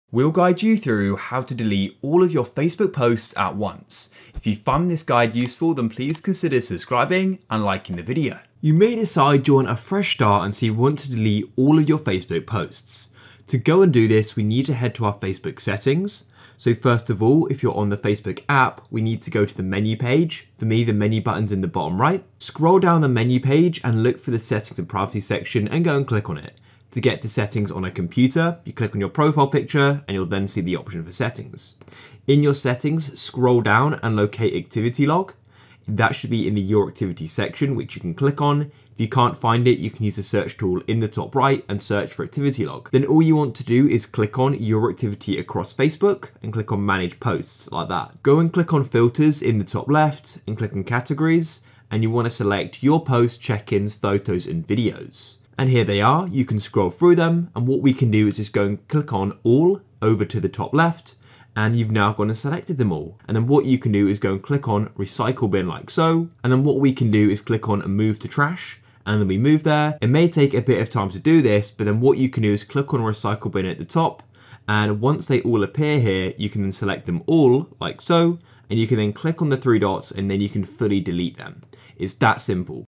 How To Delete All Posts On Facebook At Once - Full Guide
Learn how to delete all posts on facebook at once in this video.

GuideRealm